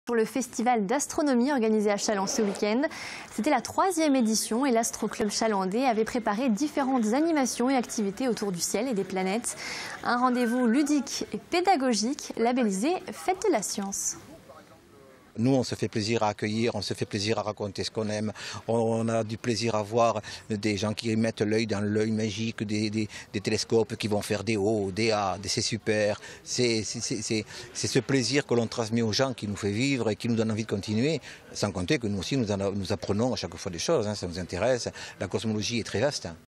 Le Festival d'Astronomie de Challans 2016 (Vendée)
Sites webs/blogs, copiez cette vidéo avec le bouton "Intégrer" !
 Retour sur la 3ème édition du "Festival d'Astronomie de Challans" organisée par l'Astro-Club Challandais le 7 et 8 octobre 2016.